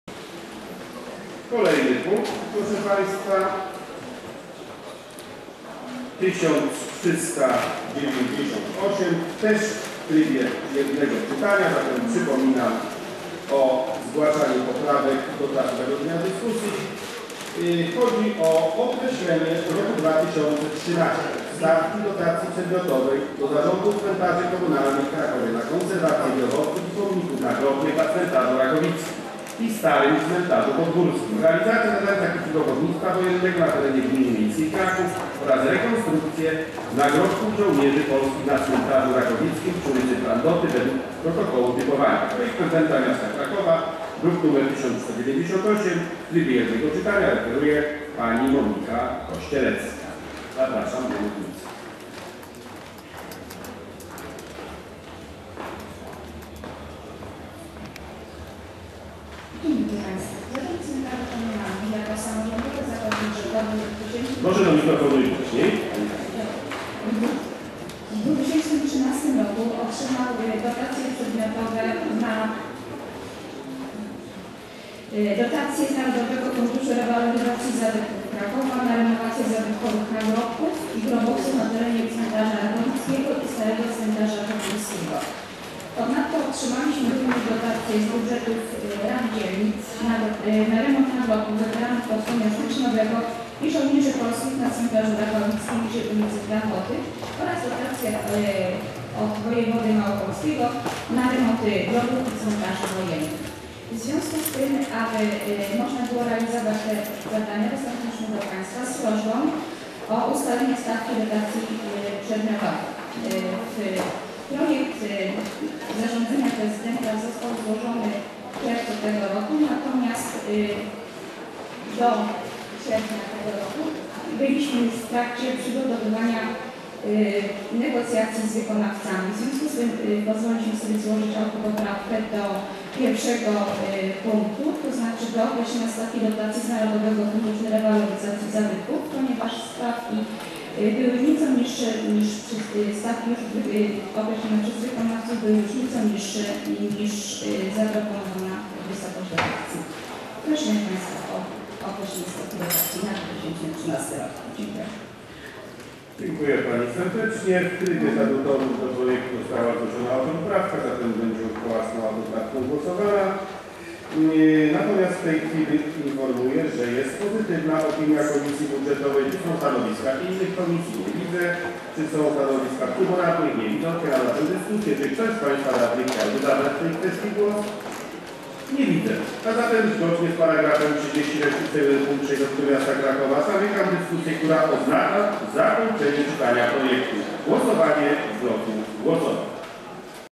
15. Stawka dotacji w 2013r. dla Zarządu Cmentarzy Komunalnych na konserwację
Określenie w roku 2013 stawki dotacji przedmiotowej dla Zarządu Cmentarzy Komunalnych w Krakowie na konserwację grobowców i pomników nagrobnych na Cmentarzu Rakowickim i Starym Cmentarzu Podgórskim, realizację zadań z zakresu grobownictwa wojennego na terenie Gminy Miejskiej Kraków oraz rekonstrukcję nagrobków żołnierzy polskich na Cmentarzu Rakowickim przy ul. Prandoty wg protokołu typowania
Projektodawca Prezydent Miasta Krakowa, referuje Monika Kościelecka
Poprawki i autopoprawki
Autopoprawka - zgłoszona przez: Prezydent Miasta Krakowa Doręczenie - 13/08/27